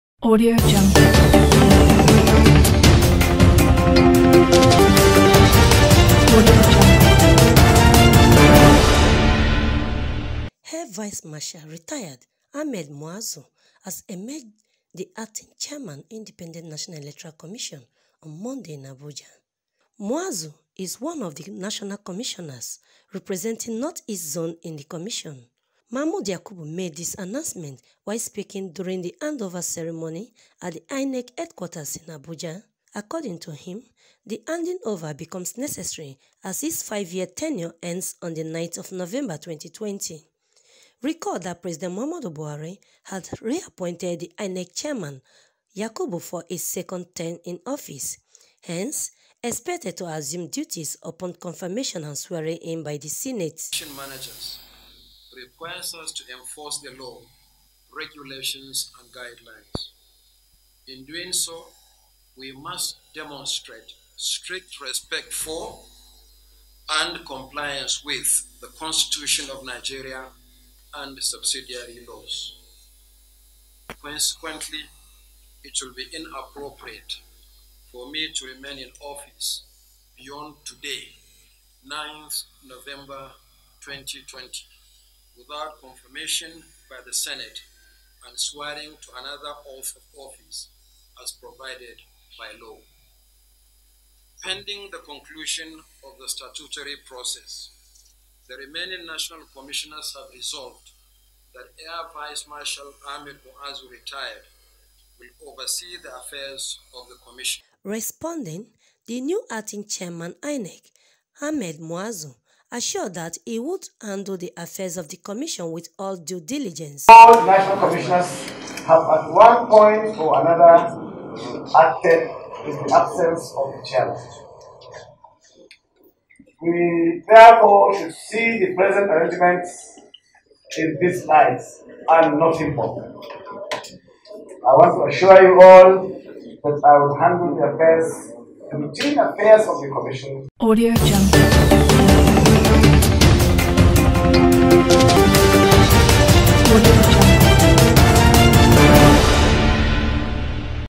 BREAKING: AIR VICE MARSHAL (RTD) AHMED MU'AZU EMERGES INEC ACTING CHAIRMAN
PROFESSOR MAHMOOD YAKUBU OFFICIALLY HANDOVER TO MU'AZU PENDING SENATE'S CONFIRMATION OVER 2ND TERM AS INEC CHAIR